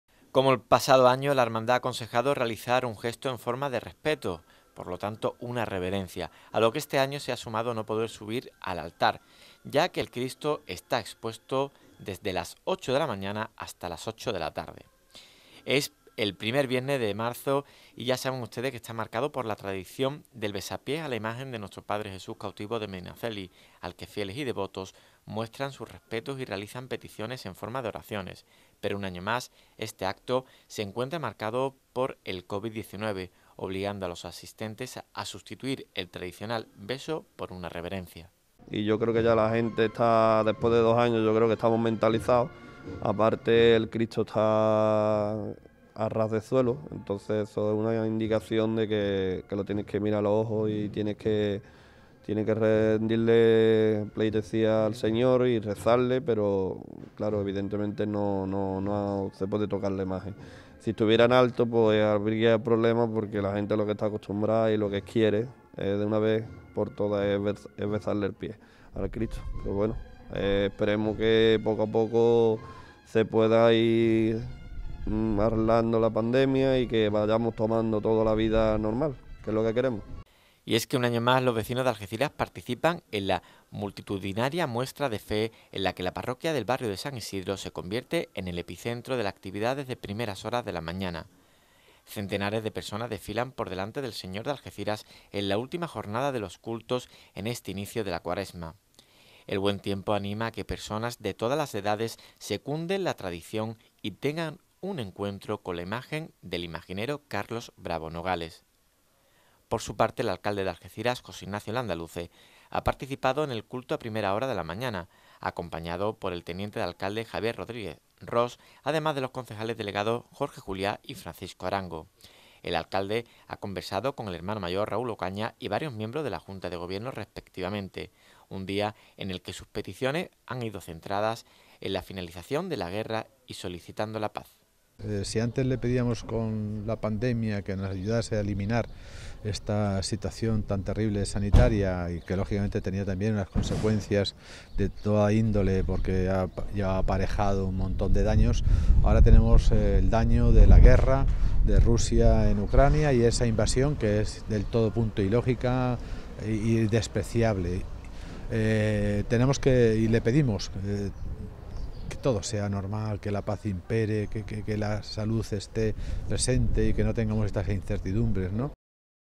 El besapiés del Medinaceli marcado por el Covid-19.
Algeciras se ha reencontrado este viernes, 4 de marzo, con Jesús Cautivo de Medinaceli dos año después del inicio de la pandemia. La hermandad del Martes Santo está celebrando en el día de hoy una solemne veneración de los fieles, concretamente una reverencia, en sustitución del tradicional besapié, debido a las condiciones higiénico sanitarias